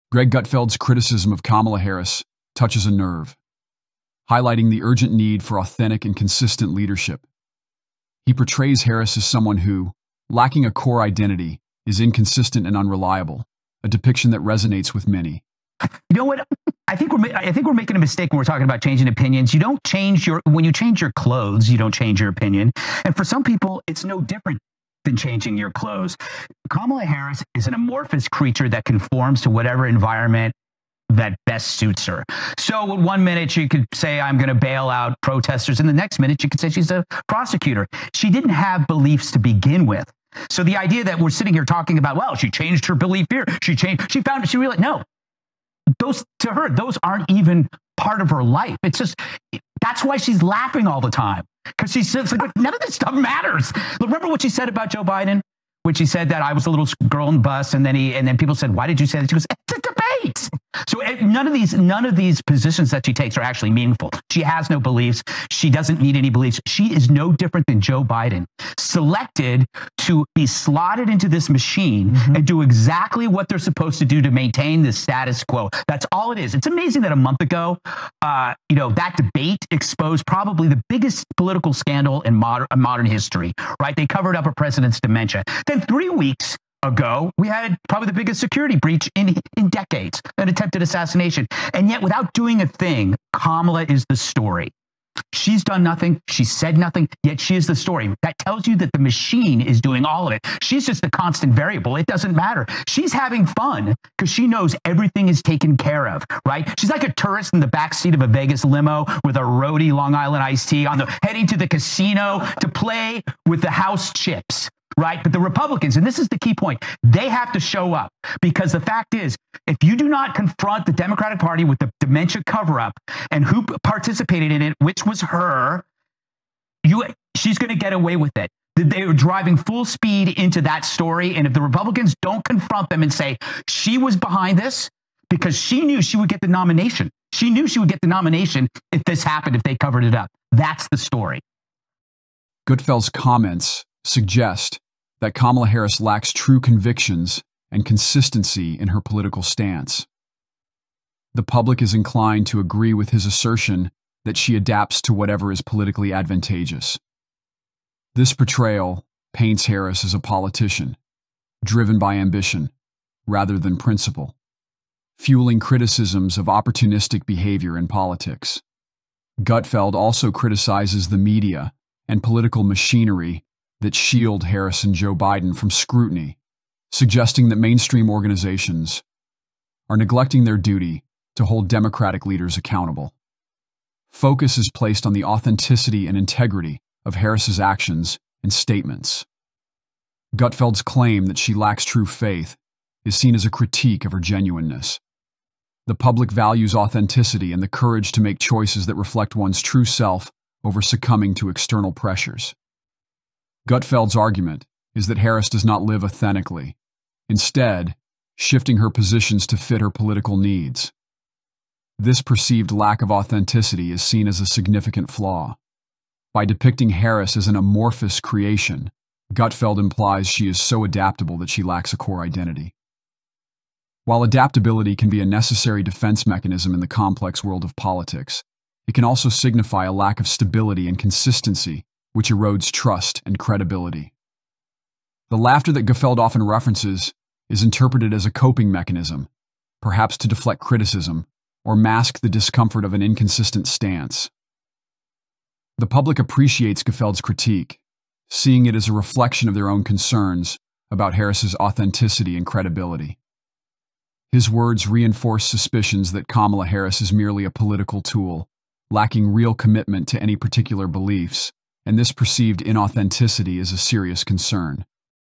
Greg Gutfeld, Kamala Harris as an Amorphous Creature Without True Beliefs
Greg Gutfeld: Kamala Harris's True Nature Exposed! In this video, Greg Gutfeld delves into the real persona of Kamala Harris, discussing her lack of consistent beliefs and her tendency to conform to any environment that best suits her ambitions. Gutfeld highlights instances from Harris's political career, shedding light on her chameleon-like behavior and questioning the authenticity of her positions. He also addresses the broader implications of such political maneuvers in the current landscape. Join us for an eye-opening discussion that reveals a side of Kamala Harris you might not have seen before. Don't miss out on this in-depth analysis!